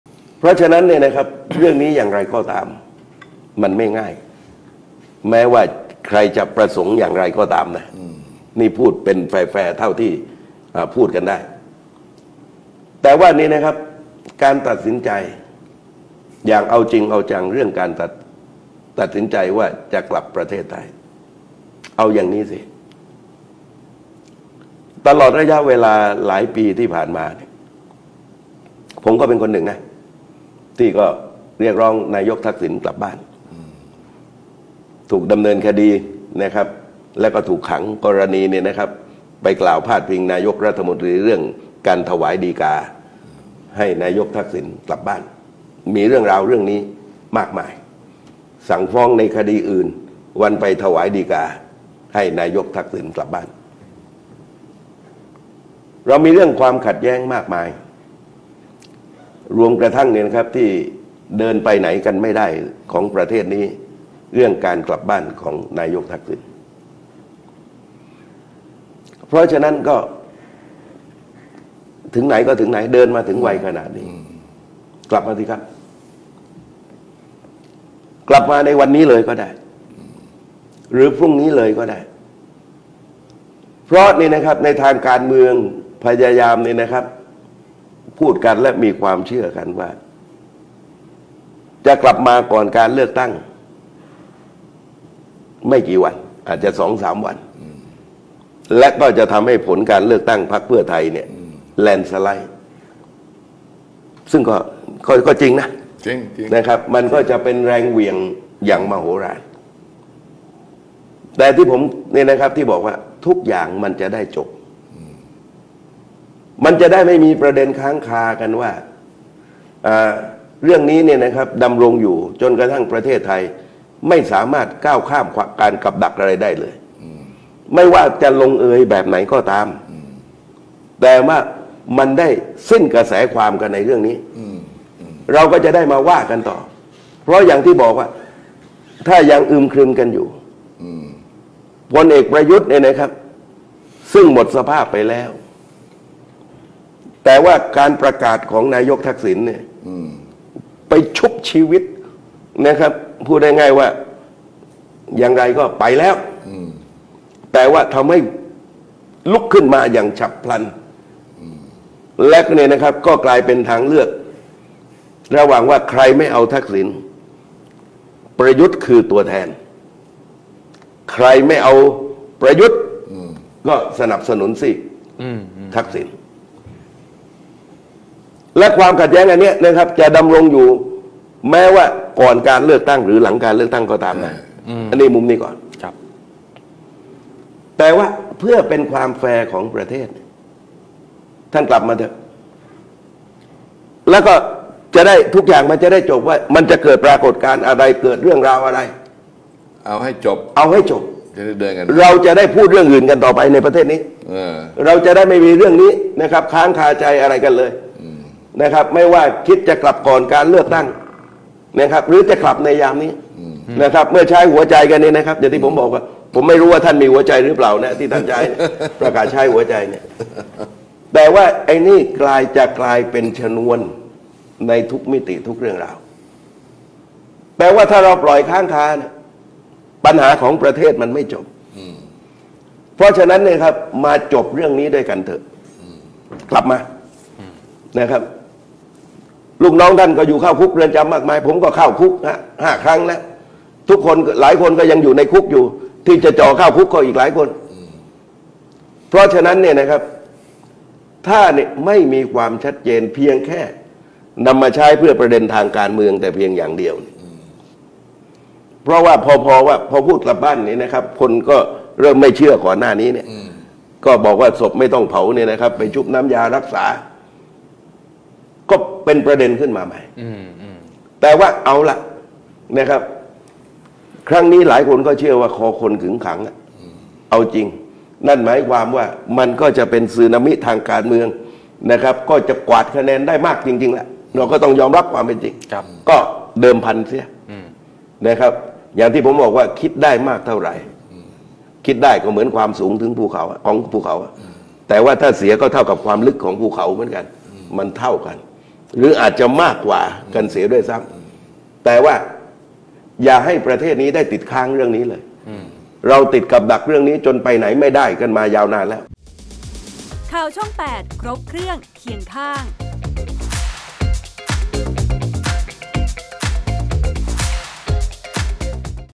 เอาแบบนี้มั้ยทักษิณ เรื่องจะได้จบๆ | ข่าวช่อง8
จตุพร พรหมพันธุ์ ท้า "ทักษิณ ชินวัตร" ทำสิ่งนี้เลยบัดเดี๋ยวนี้ เรื่องจะได้จบๆ ประเทศจะได้เดินหน้าต่อ